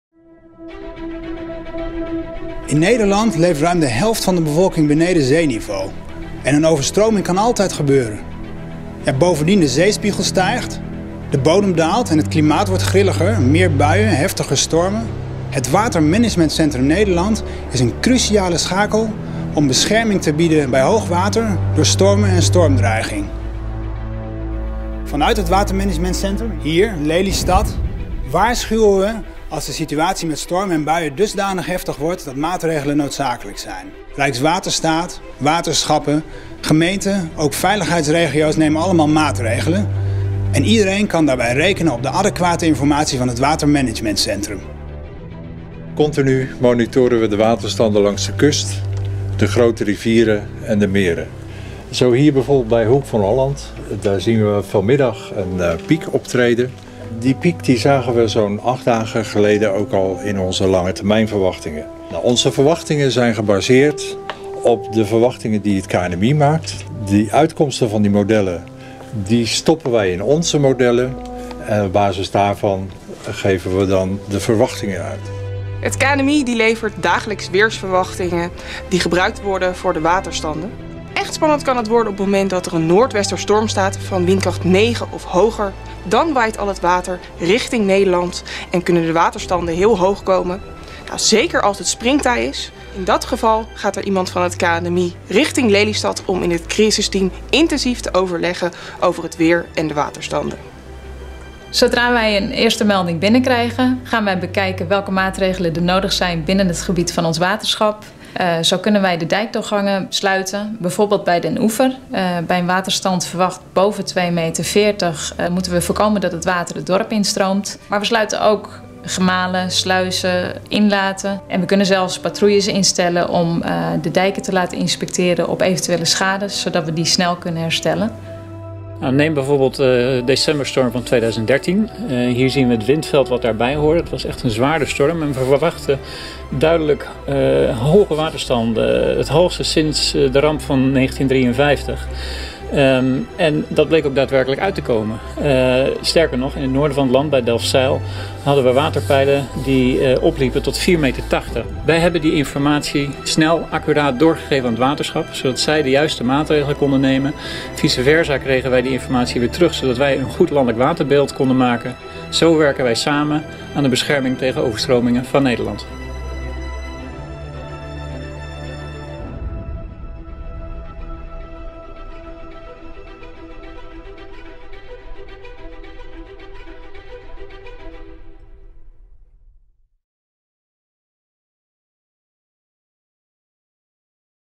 Hoe voorkomen we dat Nederland overstroomt?
Medewerkers van het Watermanagementcentrum Rijkswaterstaat, het KNMI en Hoogheemraadschap Noord-Holland vertellen hoe zij door samen te werken Nederland veilig houden bij hoogwatersituaties.